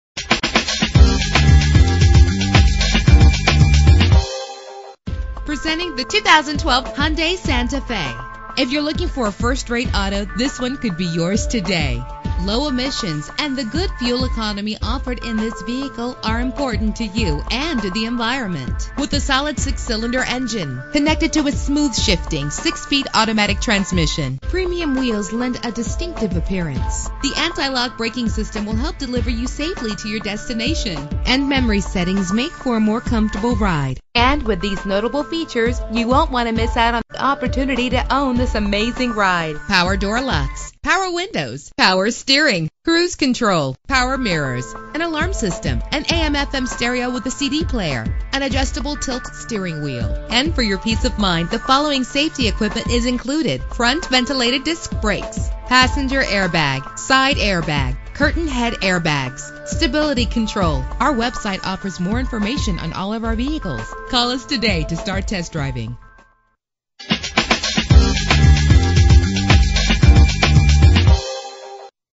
2012 Hyundai Santa Fe - Daphne AL
Year: 2012
Make: Hyundai
Model: Santa Fe
Trim: SE
Engine: 3.5 liter 6 cylinder 24 valve
Transmission: 6-Speed Automatic
Color: Glacier White
Mileage: 3
Address:
29736 Frederick Blvd.